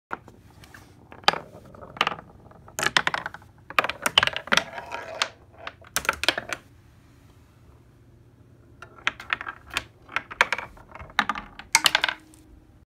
Zuzu and the Marble Run
A cat plays with a marble run in Dan Wahl's art studio.